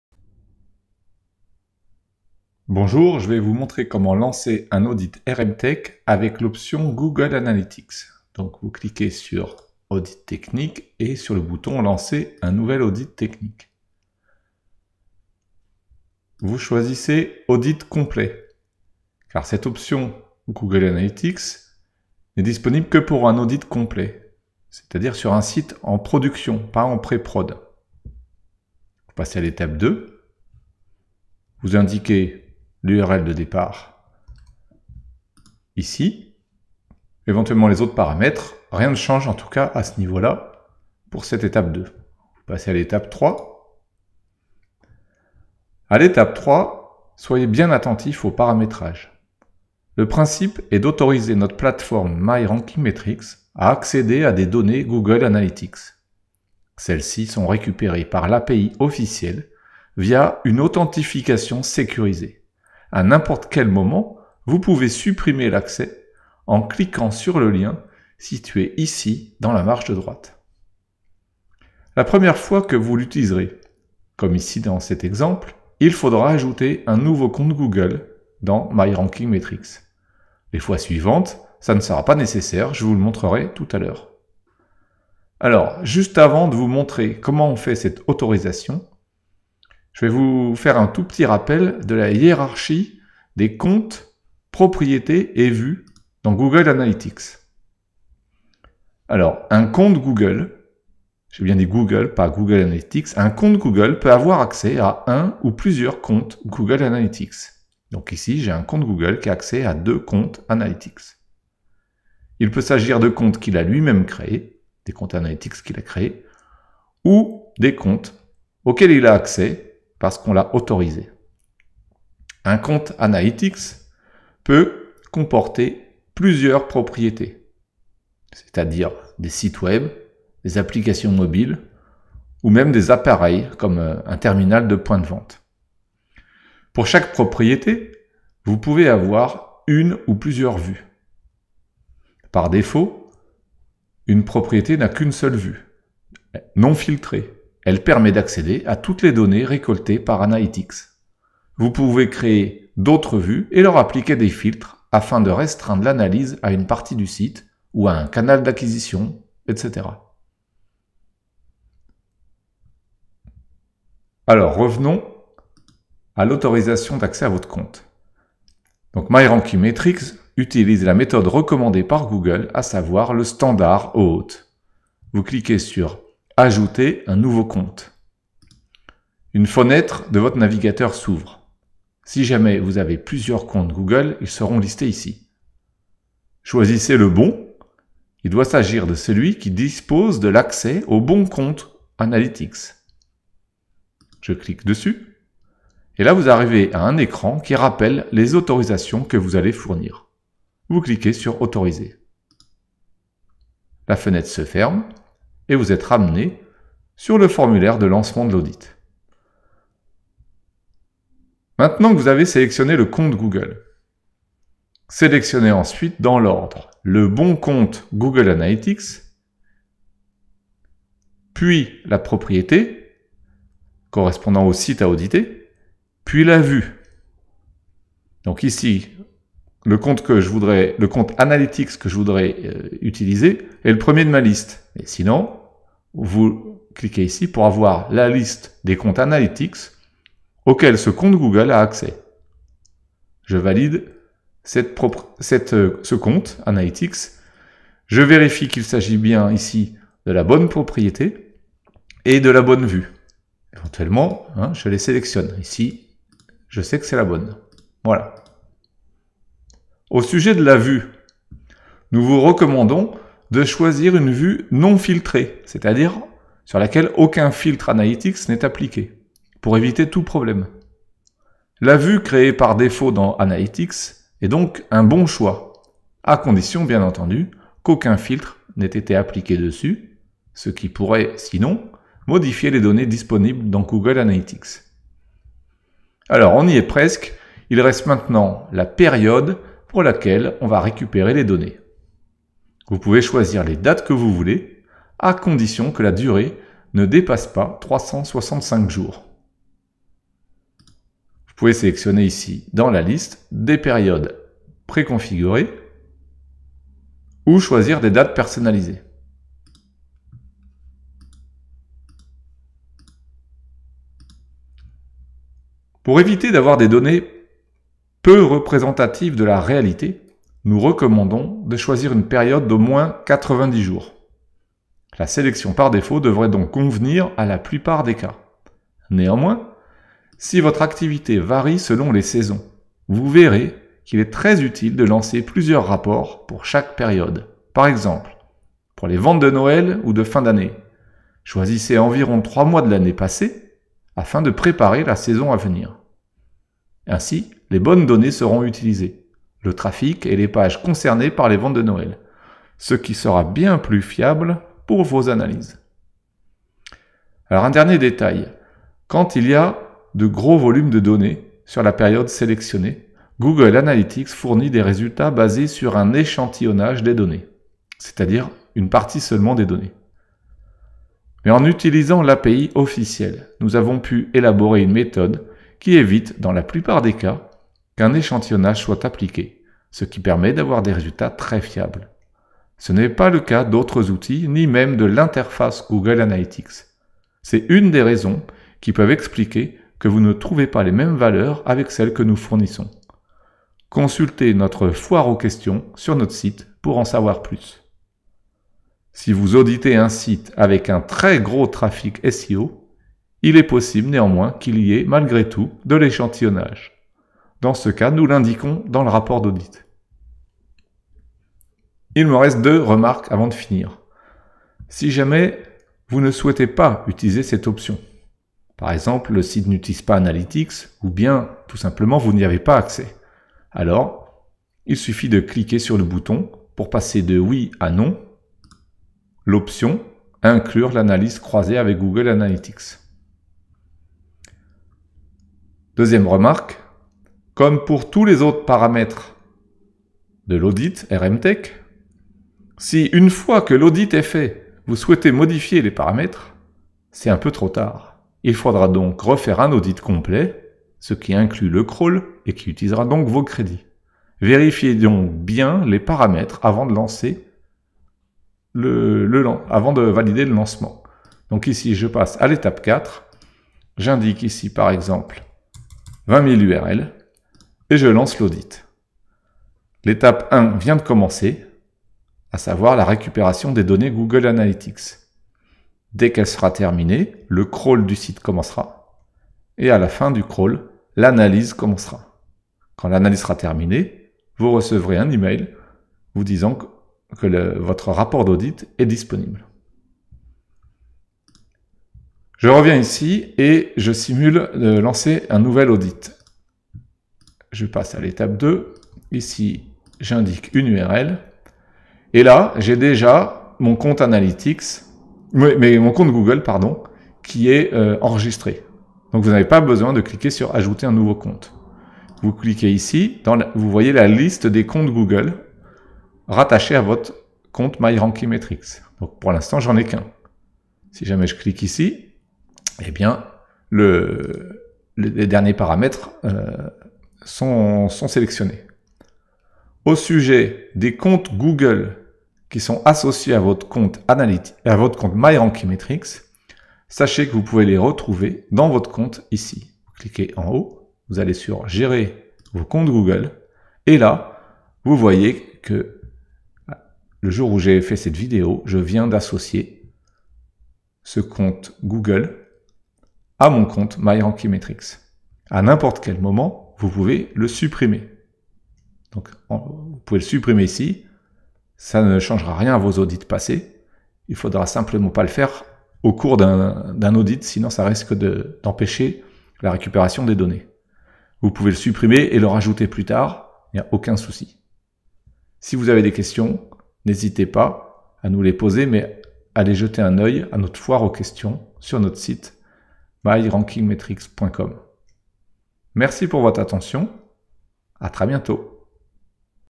Comment lancer un audit RM Tech couplé à Google Analytics
Suivez ce tuto pour configurer le lancement d'un audit RM Tech avec couplage des données Google Analytics.